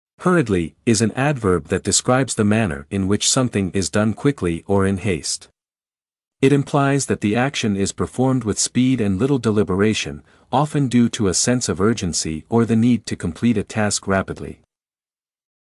What does 'Hurriedly' mean?
Discover the definition, meaning, and correct pronunciation of the word "hurriedly" in English. Perfect your language skills with our concise guide. Watch now!